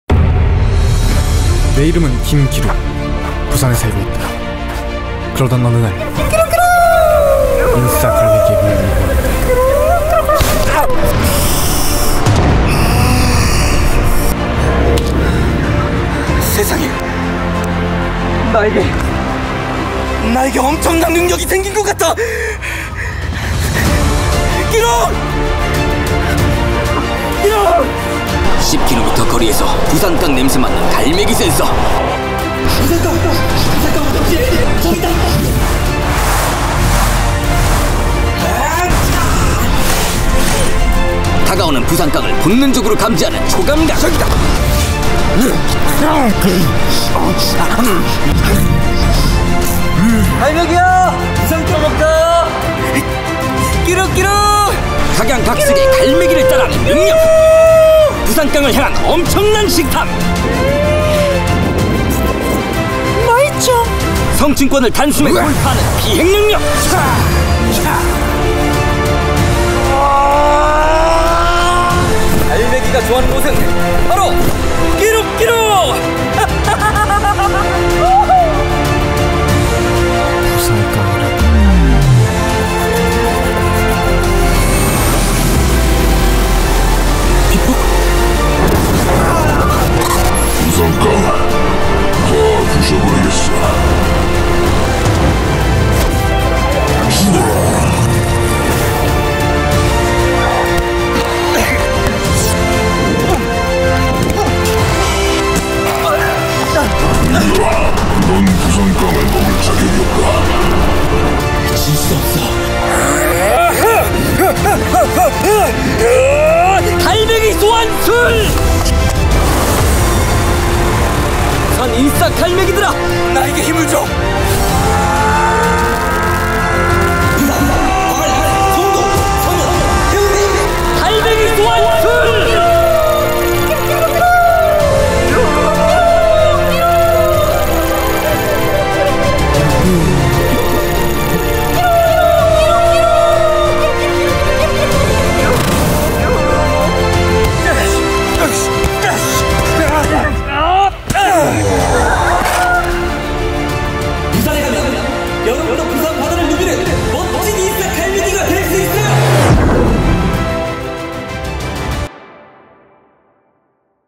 이보다 병맛은 없다! 부산에서 펼쳐지는 병맛 대결! 인싸갈매기맨 vs 부산깡 악당 과연 승자는?
부산에 사는 평범한 남자 김기룩! 어느 날 인싸갈매기에게 물려 초인적인 힘을 가진 인싸갈매기맨이 된다.

부산깡을 연구하다 피폭 당해 부산깡을 혐오하는 악당으로 변해버린 부산깡 악당.
부산깡을 좋아하는 인싸갈매기맨을 처치하기 위해 밖으로 나오고
해수욕장에서 인싸갈매기맨과 조우하게 되는데..

부산 해수욕장에서 펼쳐지는 병맛 대결!
과연 승자는 누구인가!?

인싸갈매기맨 VS 부산깡 악당 영상은 부산 인싸갈매기 이벤트 참여자들의 사진을 활용하여 제작되었습니다.